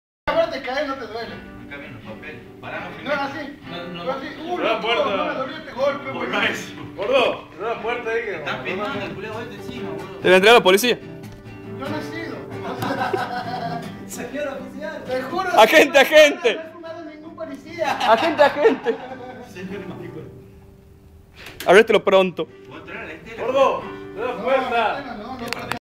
Asado en lo del Mono - Enero 2009 video 1
Asado y pileteada en lo del Mono Sabado 24/01/09